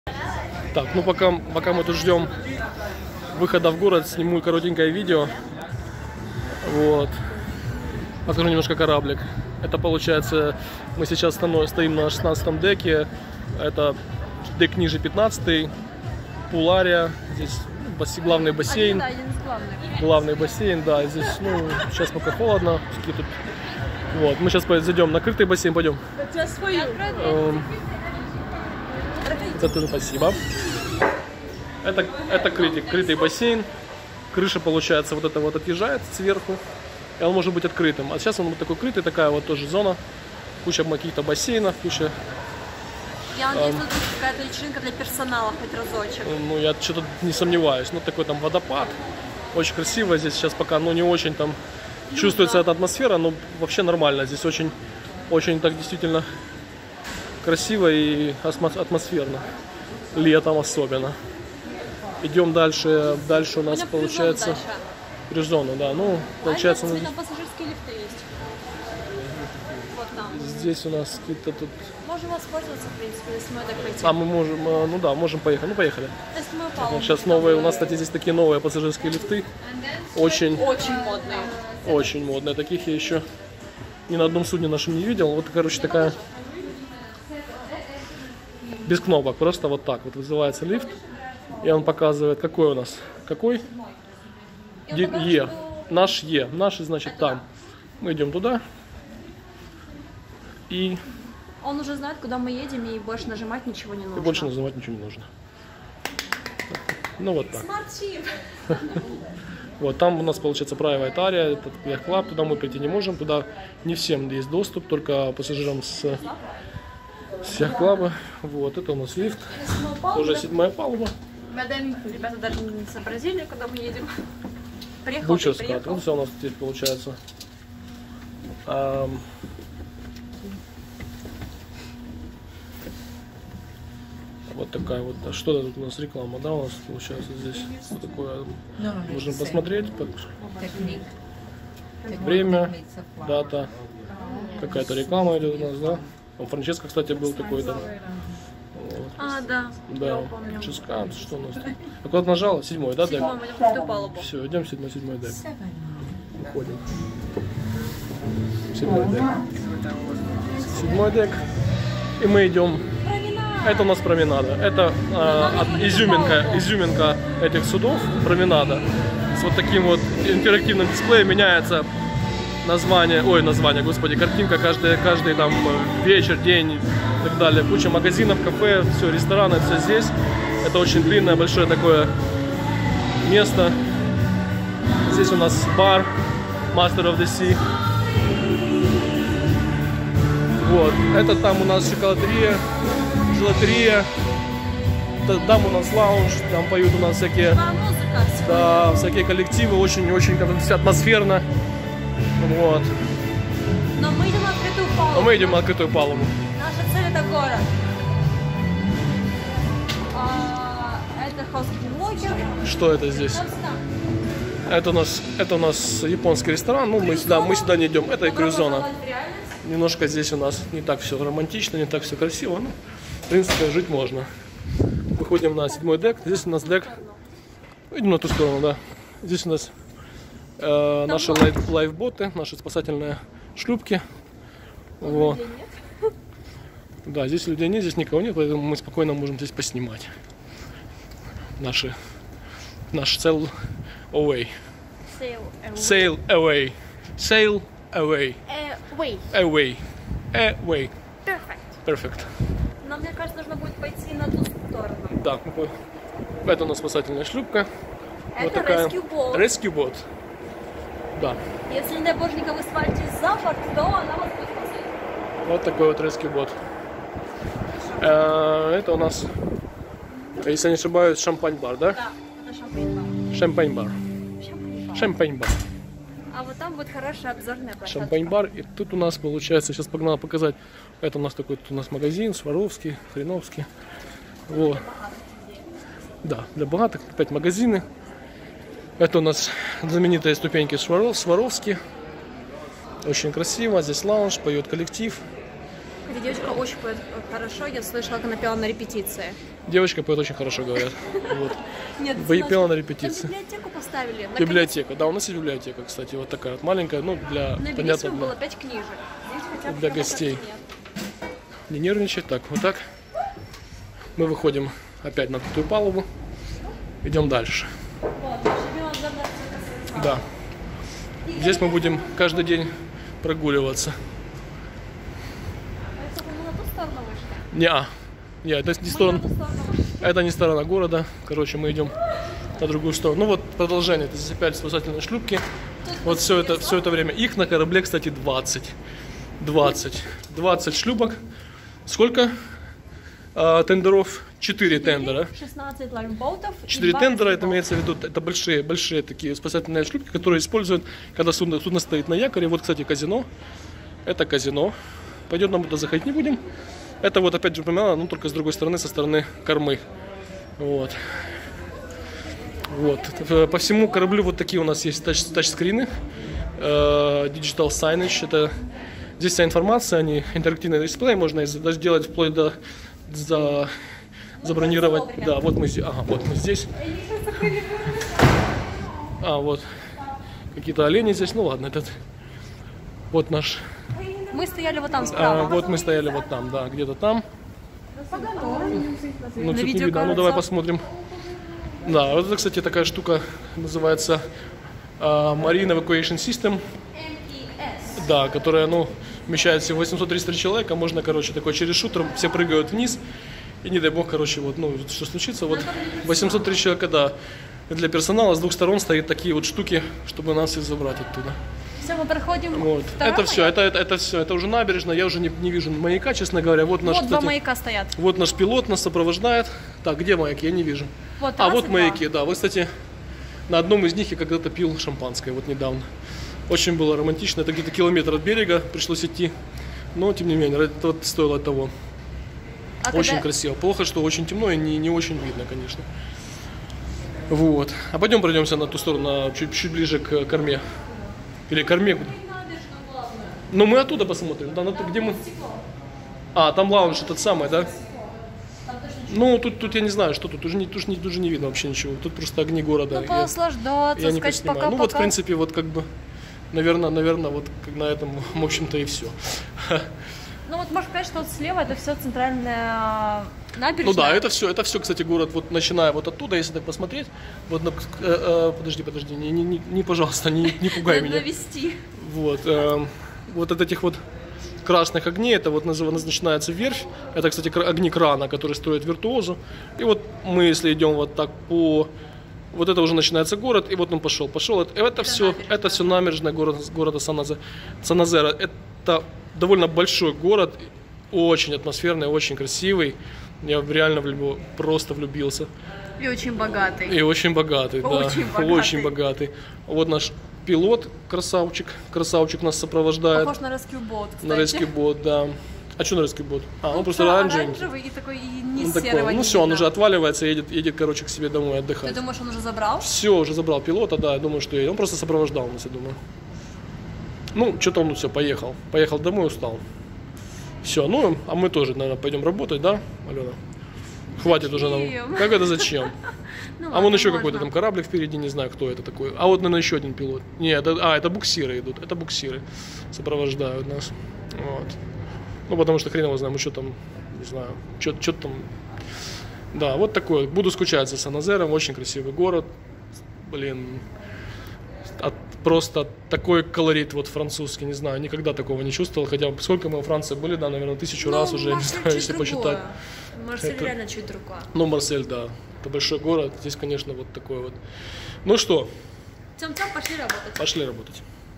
First voyage | MSC Grandiosa leaves the SHIPYARD
The newest ship MSC Grandiosa cruise ship furst time sails away from  Saint Nazaire shipyard. My collegue and I have made short video about this great event.
The ship uses the new generation technologies: modern interactive displays, mobile apps, augmented reality simulators, the newest medical equipment, ect.